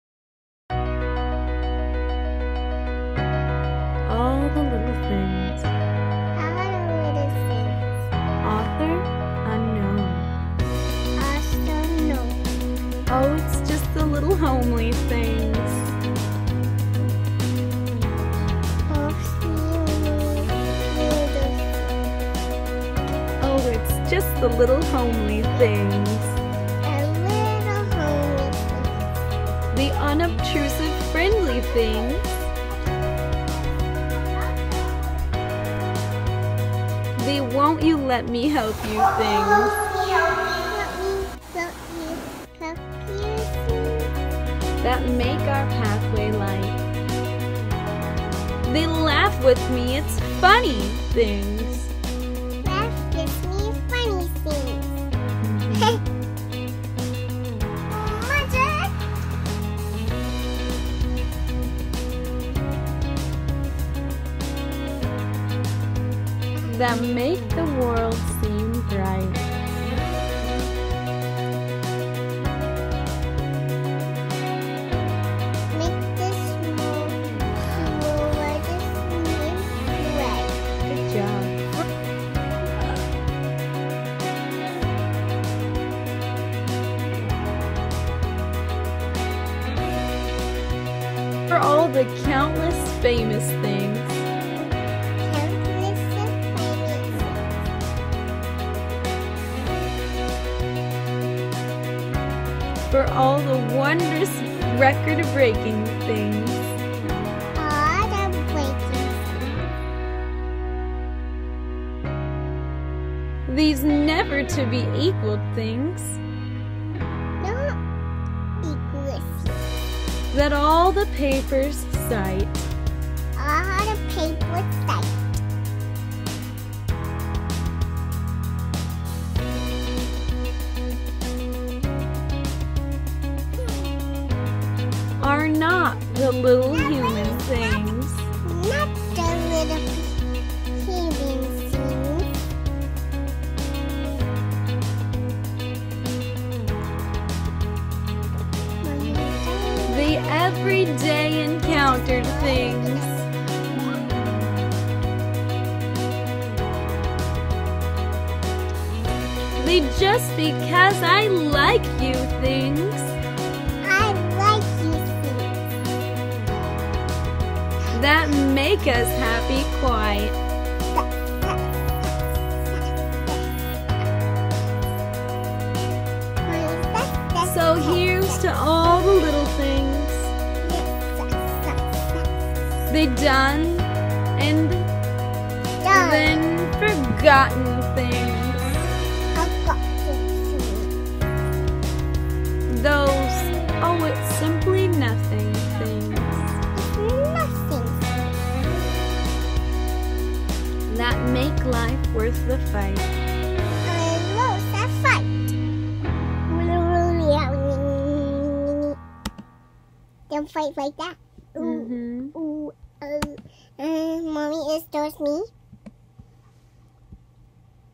All The Little Things Inspirational Poetry Humble
Let's take a moment to appreciate "All The Little Things" this is a poem that I found in an old book.